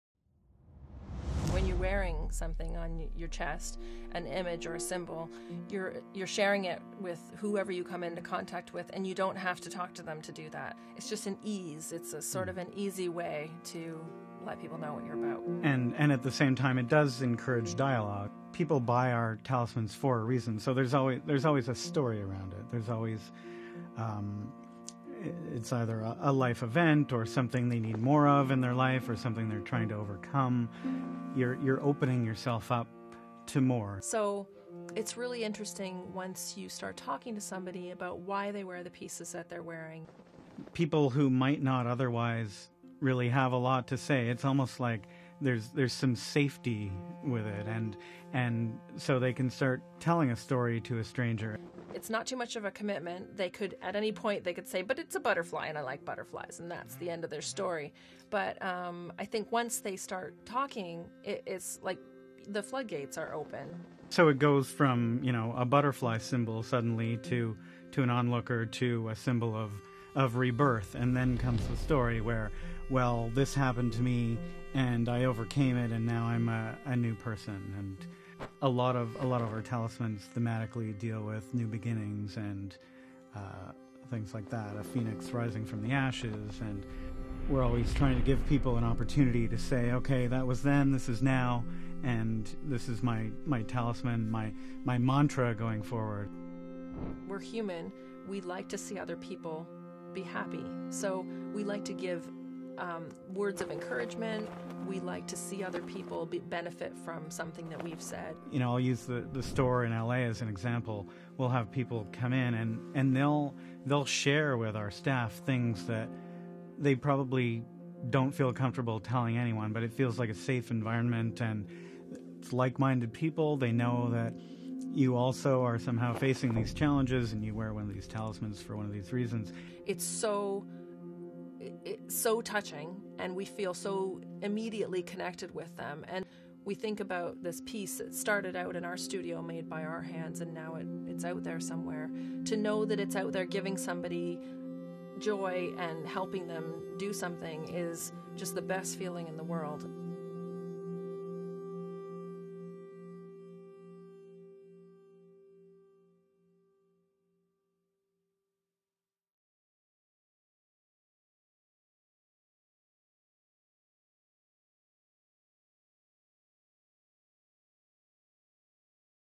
Storytelling through Jewellery ... Pyrrha | Fifth Avenue Jewellers
Pyrrha's Wade and Danielle describing everything Pyrrha in their words. amazing people, a must watch! Pyrrha talismans available at fifthavenuejewellers.com today.